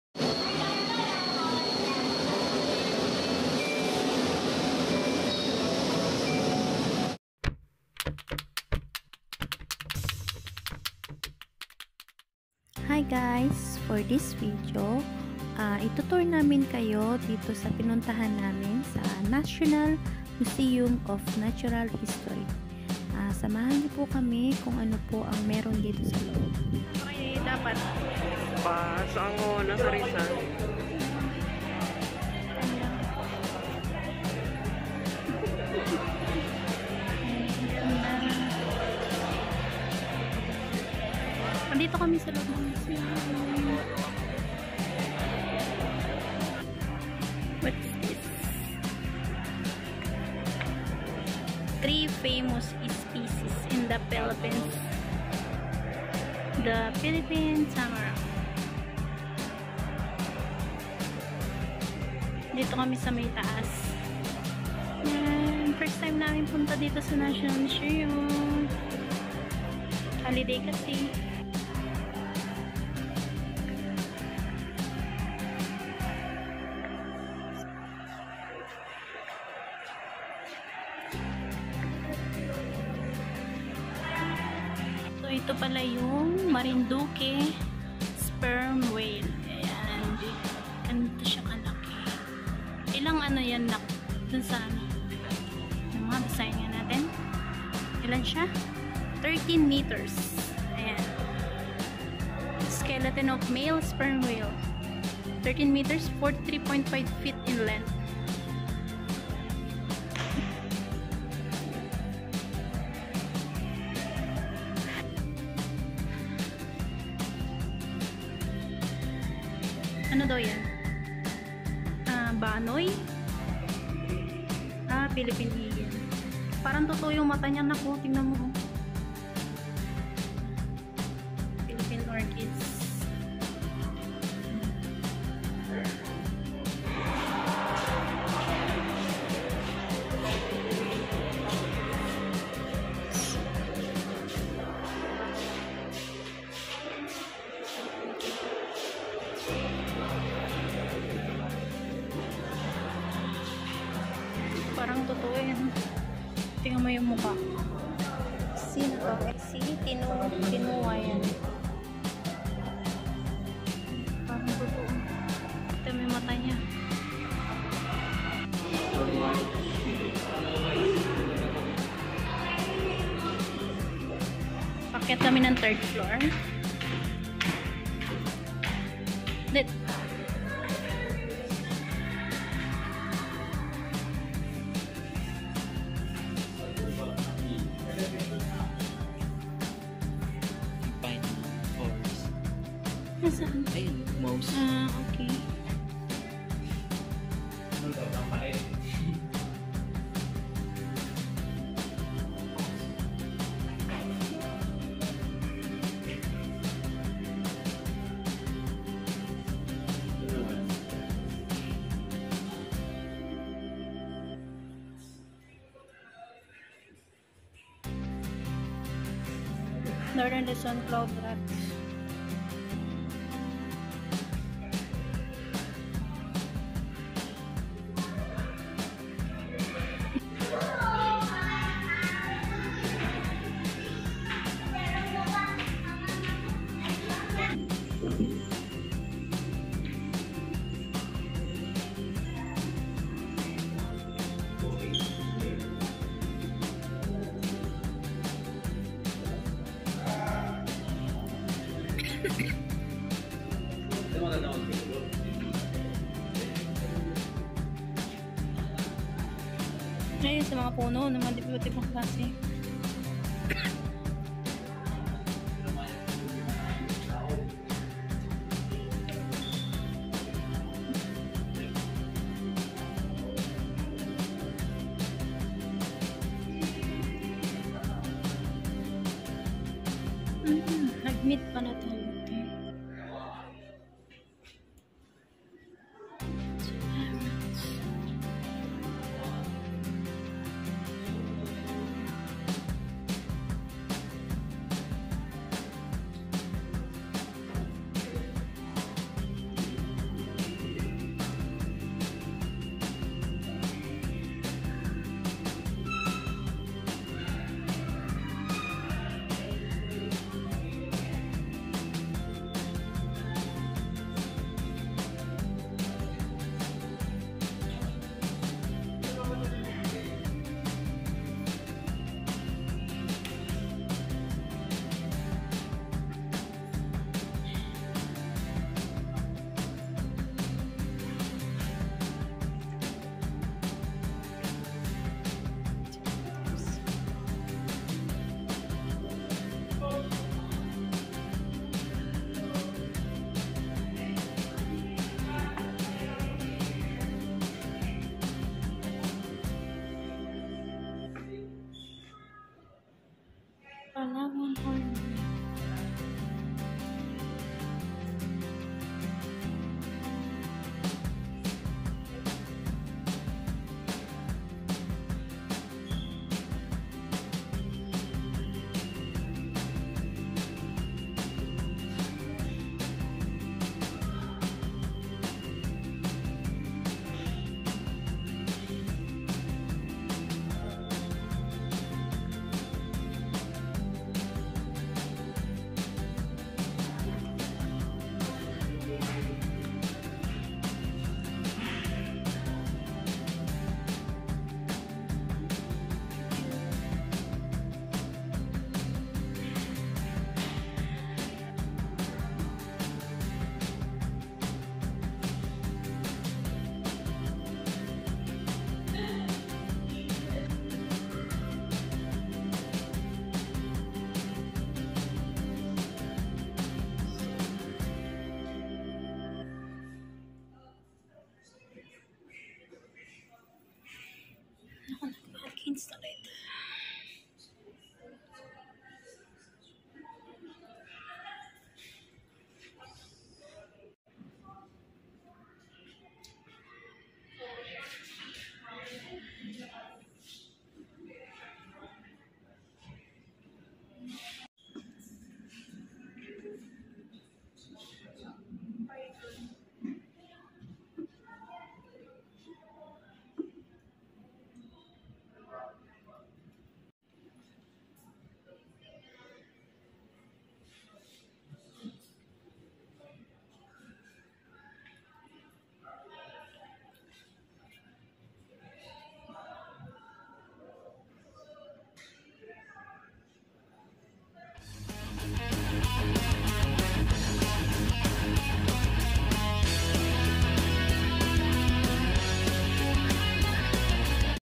NATIONAL MUSEUM OF NATURAL HISTORY PART1
Hi guys for this video share ko lang po yung experience namin noong pumunta kami diyan holiday June 12 dapat sa Luneta lang kami pupunta kaso na curios kami kung anong meron doon sa national museum of natural history.